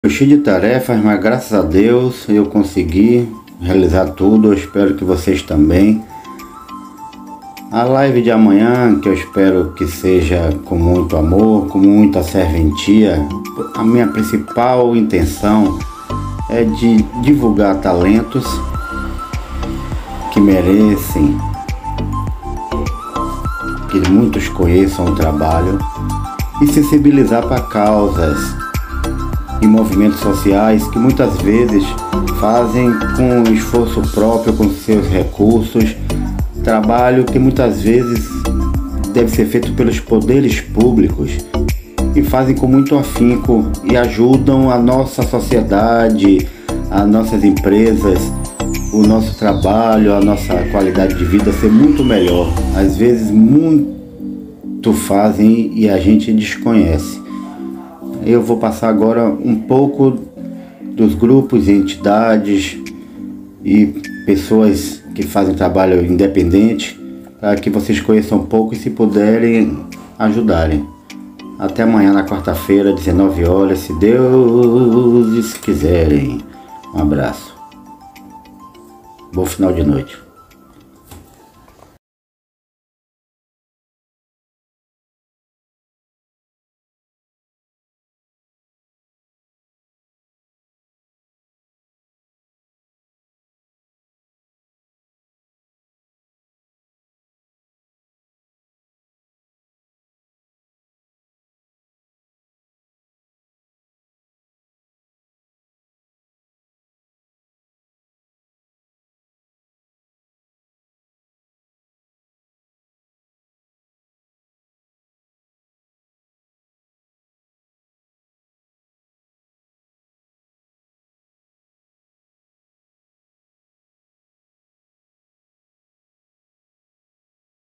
Ajudar com entidades e pessoas físicas. Sobre o Papo entre Tagarelóides de 27/10/2021
Papo entre Tagarelóides

- Ufologia;
- Festival de Jazz na Chapada Diamantina;
- Asfalto no Capão;
- Combate a incêndios florestais;
- Causa da Proteção animal;
- Músicas e Etí cetras...